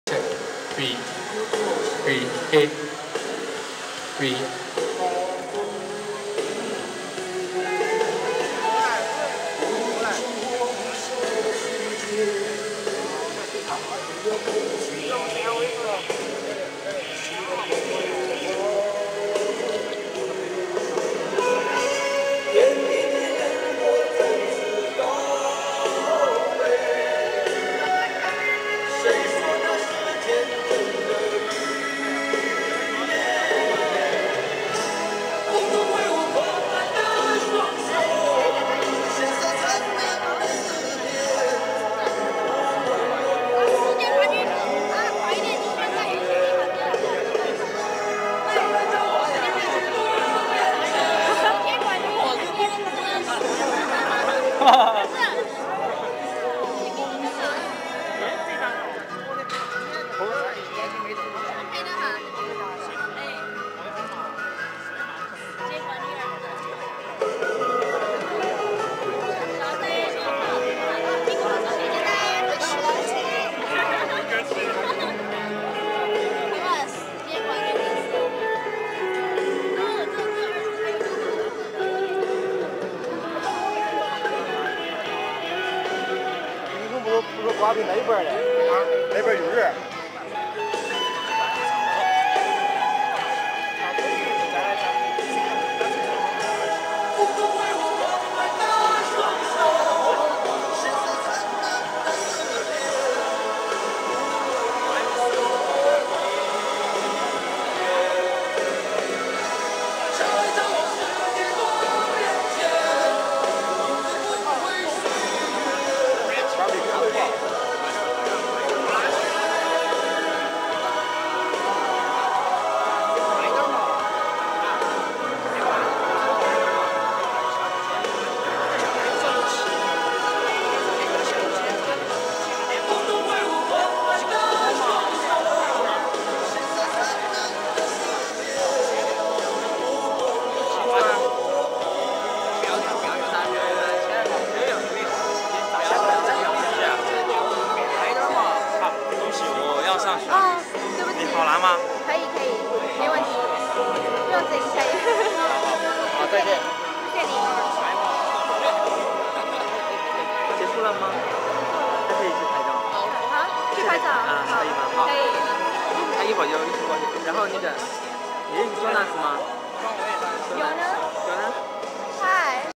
IEM Chengdu - October 2009 - Video 4
Passionate fans taking turns to take pictures with Manuel "Grubby" Schenkhuizen.